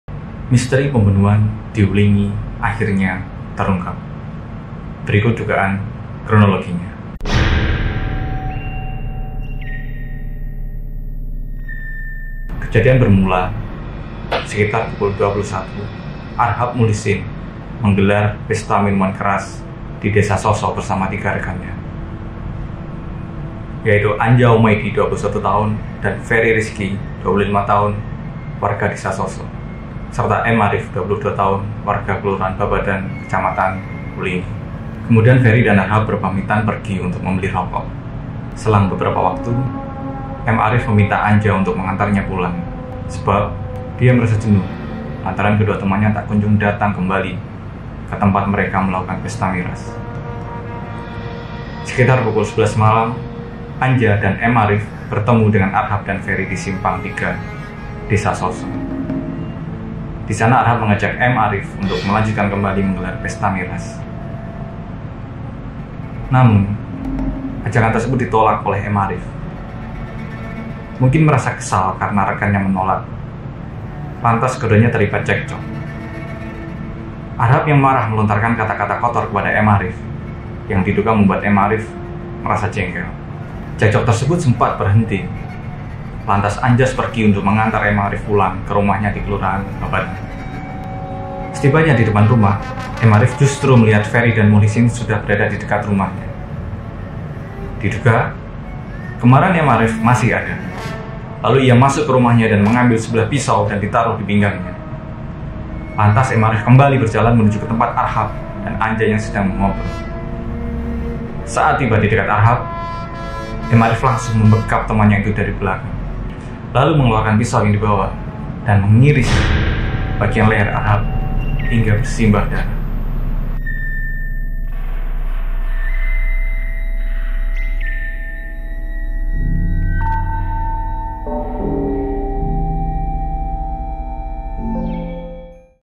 Misteri kematian pemuda di wlingi akhirnya terkuak
Kejadian bermula sekitar pukul 21.00, Arhab Muhlisin menggelar pesta minuman keras (miras) di Desa Soso bersama tiga rekannya.
Yaitu Anja Umadi, 21 dan Fery Riski, 25, warga Desa Soso; serta M. Arif, 22, warga Kelurahan Babadan, Kecamatan Wlingi.

Kemudian Fery dan Arhab berpamitan pergi untuk membeli rokok. Selang beberapa waktu,
M. Arif meminta Anja untuk mengantarnya pulang. Sebab, dia merasa jenuh lantaran kedua rekannya tidak kunjung kembali ke tempat mereka melakukan pesta miras.

Sekitar pukul 11 malam, Anja, dan M. Arif bertemu dengan Arhab dan Fery di simpang tiga Desa Soso. Di sana Arhab mengajak M. Arif untuk melanjutkan kembali menggelar pesta miras.

Namun, ajakan tersebut ditolak oleh M. Arif. Mungkin merasa kesal karena rekannya menolak, lantas keduanya terlibat cekcok. Arhab yang marah melontarkan katakata kotor kepada M. Arif, yang diduga membuat M. Arif merasa jengkel.

Cekcok tersebut sempat berhenti, lantas Anja pergi untuk mengantar M. Arif pulang ke rumahnya di Kelurahan Babadan. Setibanya di depan rumah M. Arif, justru melihat Fery dan Muhlisin sudah berada di dekat rumah M. Arif.

Diduga, kemarahan M. Arif masih ada, lalu ia masuk ke rumahnya dan mengambil sebilah pisau dan ditaruh di pinggangnya. Lantas M. Arif kembali berjalan menuju ke tempat Arhab dan Anja yang sedang mengobrol.
Saat tiba di dekat Arhab, M. Arif langsung membekap temannya itu dari belakang. Lalu mengeluarkan pisau yang dibawa dan mengiris bagian leher Arhab hingga bersimbah darah.